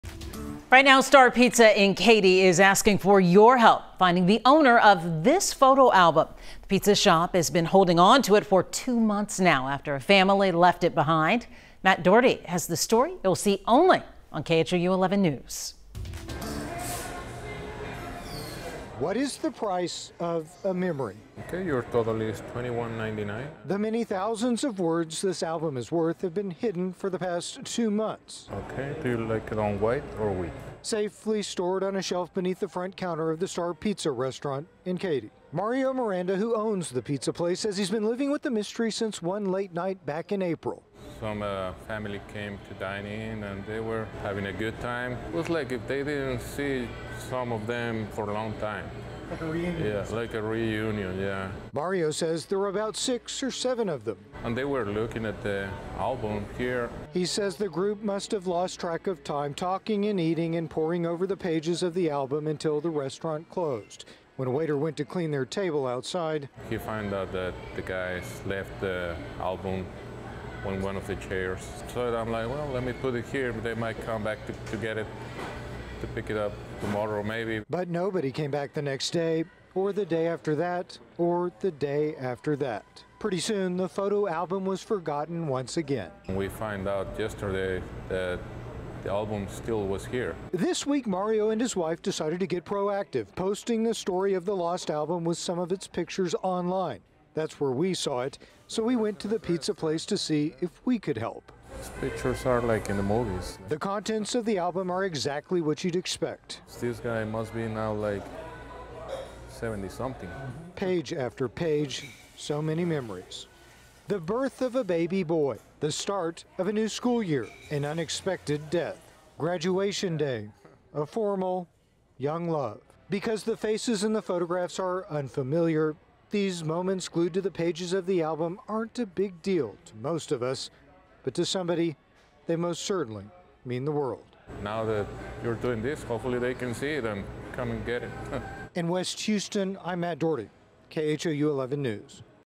Houston-area pizza restaurant searching for owner of left behind family photo album
The album contains family photos and documents spanning multiple generations.